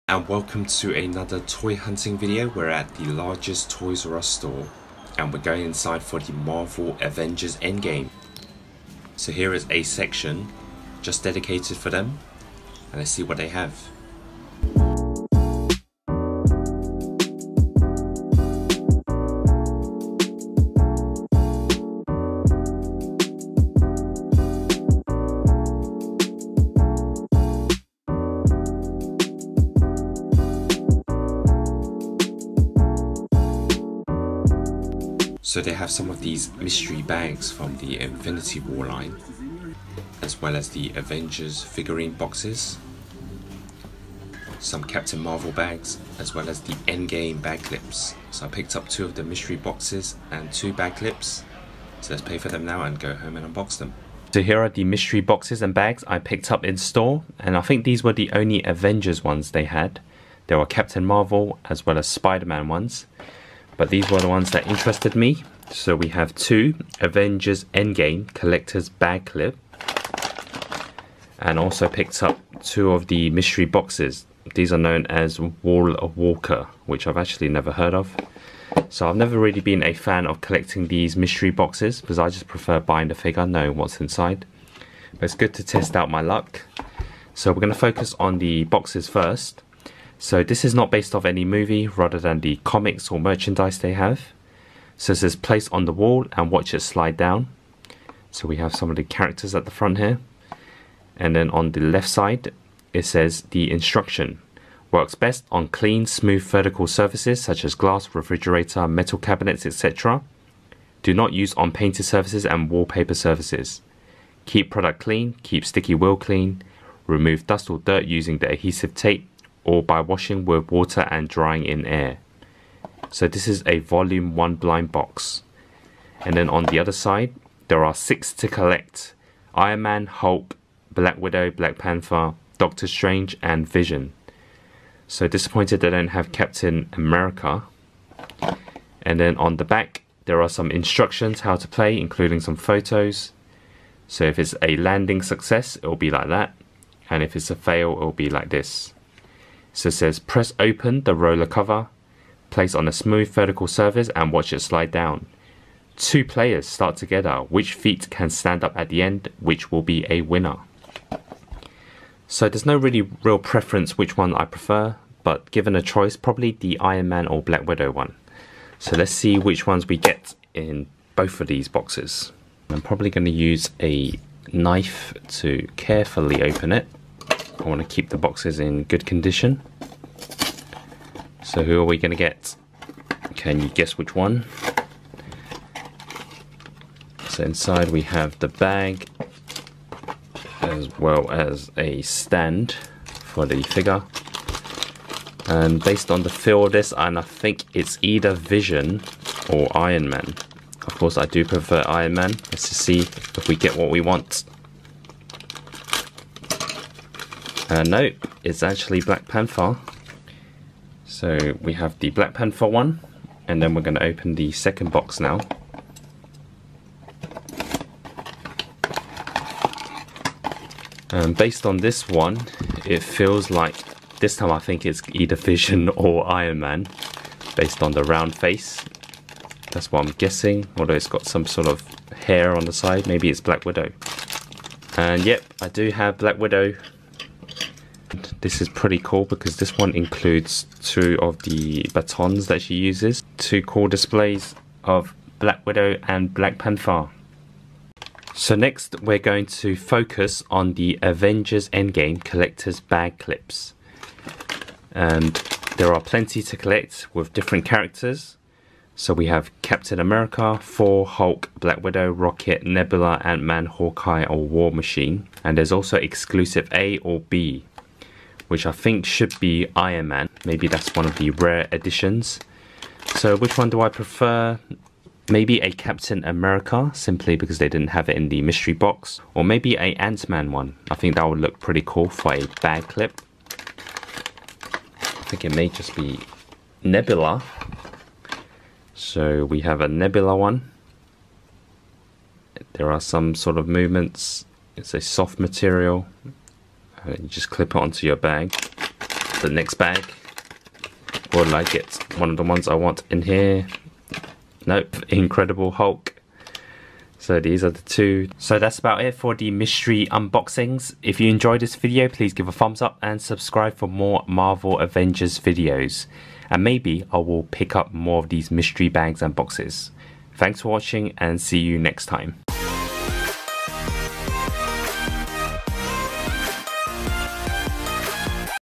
AVENGERS MYSTERY TOYS | Toys R Us pick ups
Welcome to another Marvel Avengers toy video.

Today I will show you a quick browse at a Toys R Us where they have a large selection of Marvel figures and toys. I managed to pick up some of these mystery/blind boxes and bags. And share with you what I found in them!

It’s the Avengers Wall Walker and the Endgame Collectors Bag Clip.